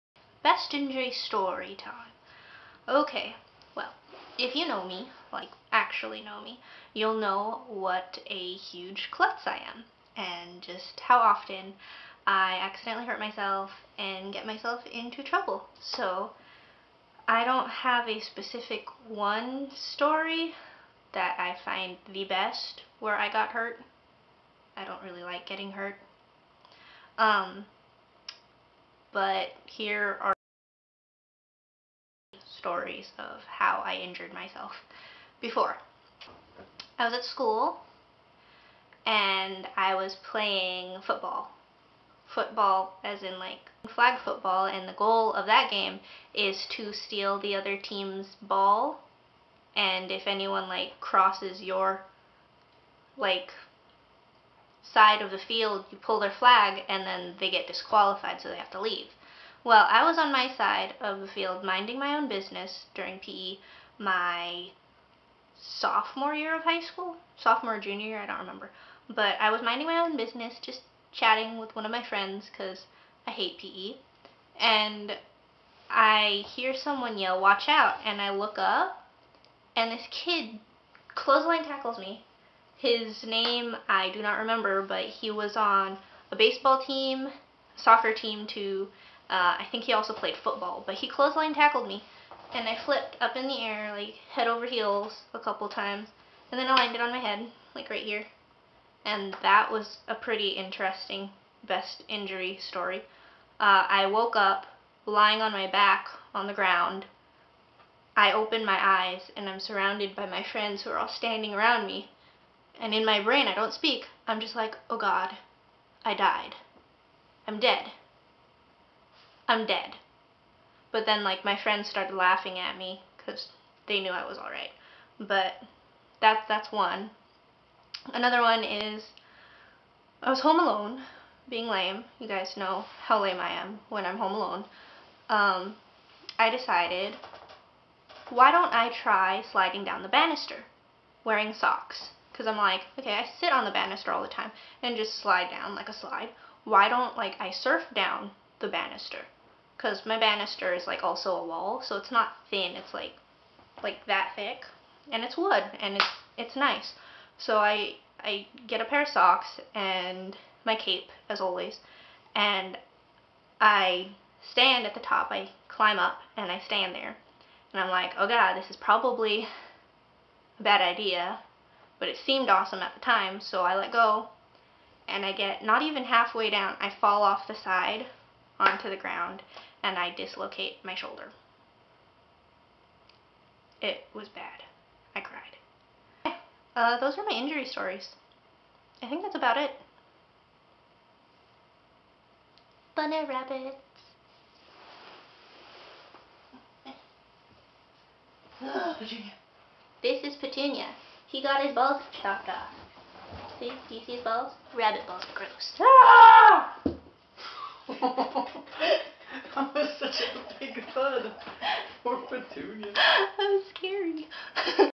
Azien's Injury Stories. WARNING Rabbit Testicles!
Azien is a klutz. She also likes animals.

Come back EVERY WEEK for a NEW THEME and THREE NEW VLOGS! :D